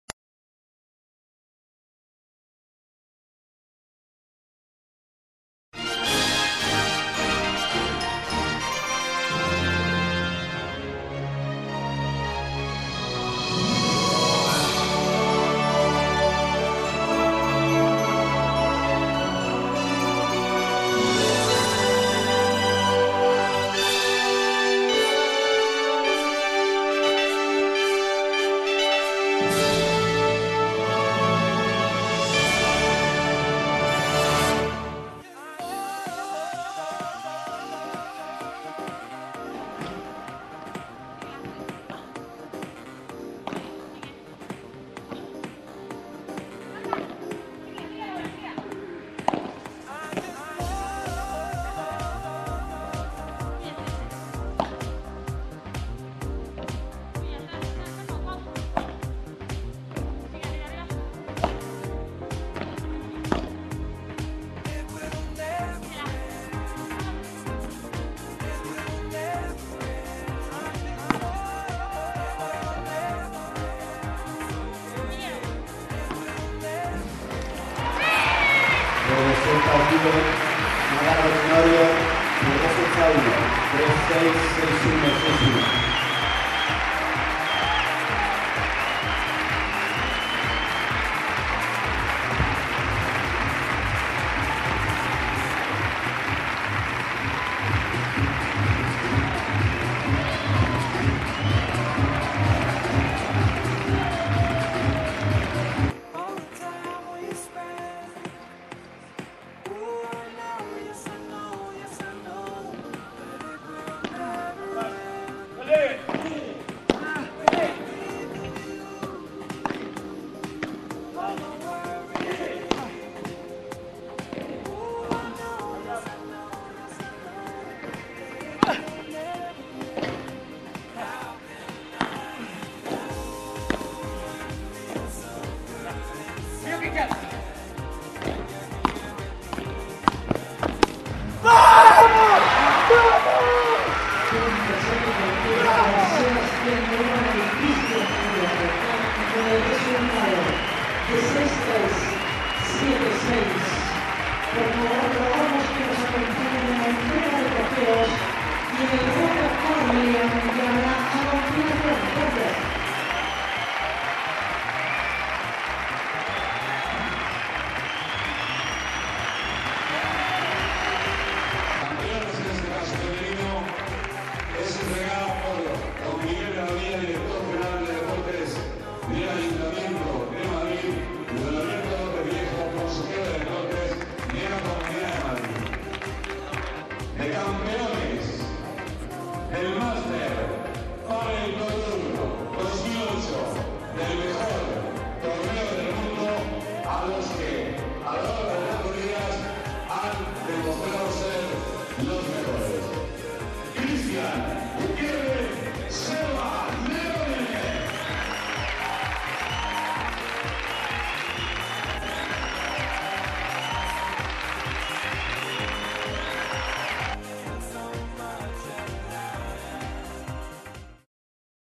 Master PPT Tour 2008 - PadelCentrum
Impressions of the Master PPT Tour 2008
to know more about Padel in Holland.